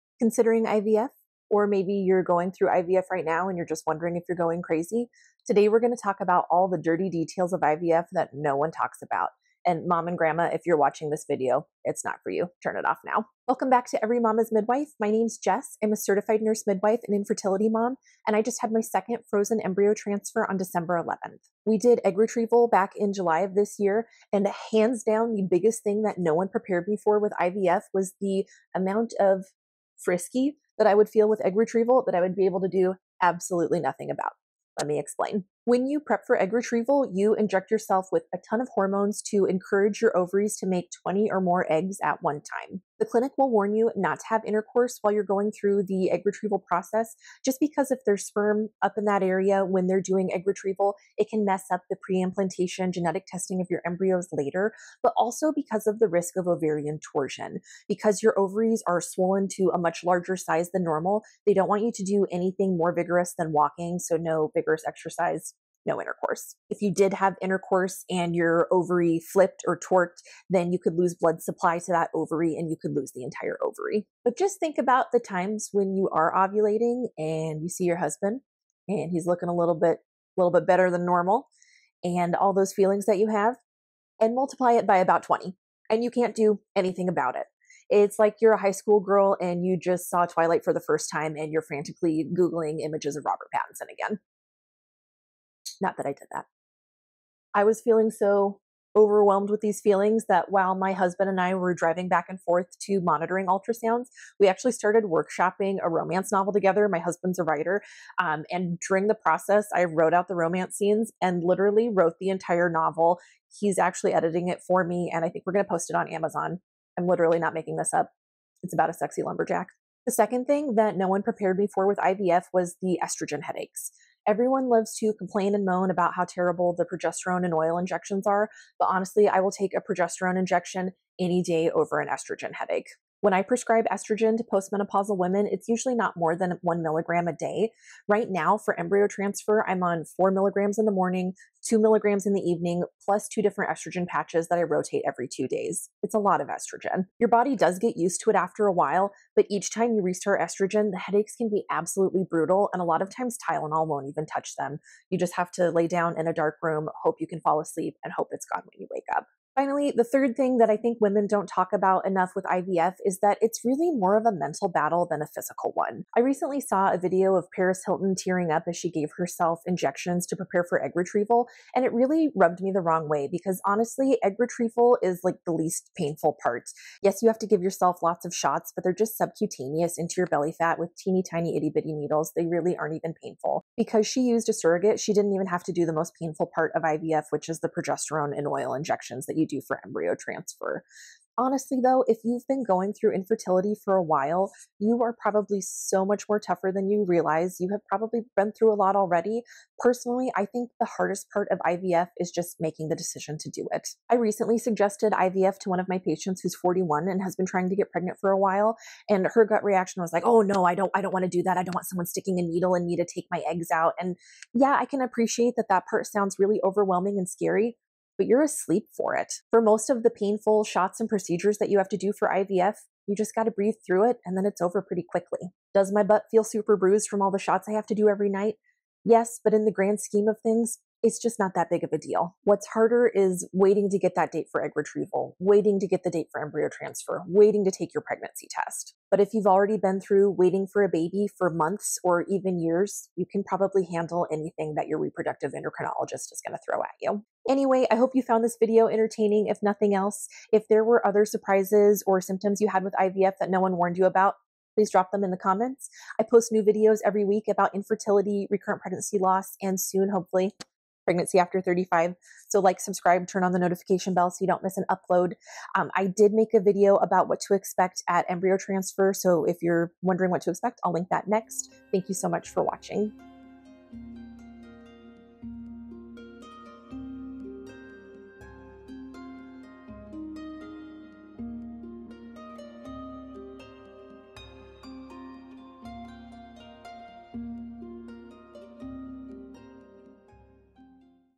Down & Dirty IVF Details!
Wondering if you're tough enough for IVF? Here's three things you can "look forward" to!

00:00 Intro
00:21 The First Thing No One Prepares You For
01:59 The Second Thing No One Prepares You For
02:39 The Third Thing No One Prepares You For
04:07 Outro